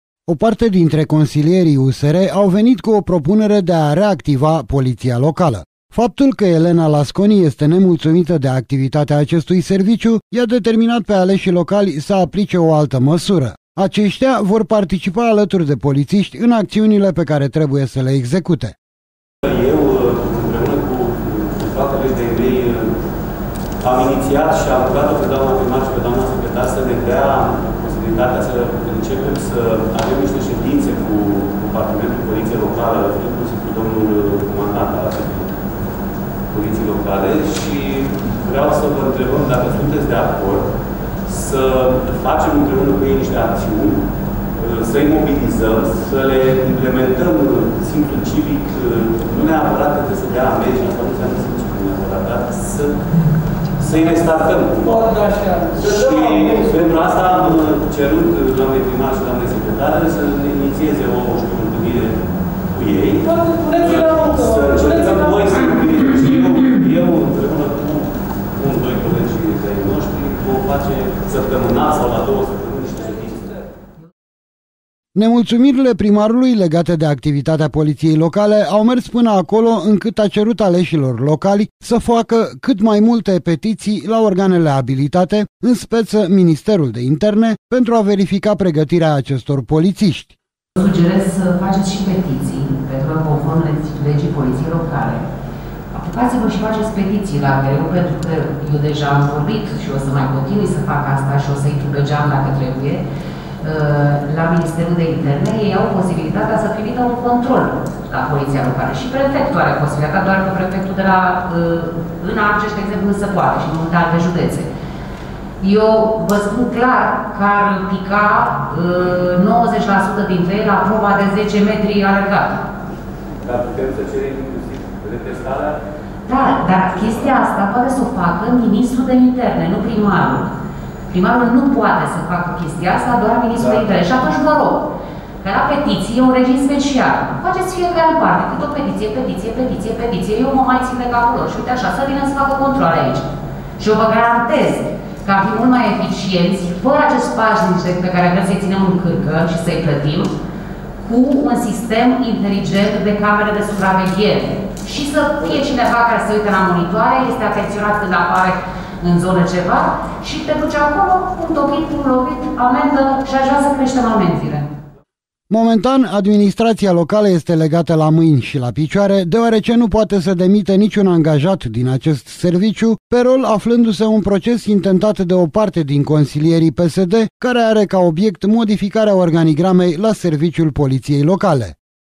Poliţia locală, un serviciu aflat la terapie ( 09 07 2021 )
Scurtă descriere:

Pentru a efienticiza serviciul poliţiei locale, o parte dintre consilieri au cerut îngăduinţa să contribuie la rezolvarea acestei probleme. Prin urmare, aceştia vor interveni alături de poliţişti acolo unde este nevoie în teren şi mai ales la sesizările importante făcute de cetăţeni.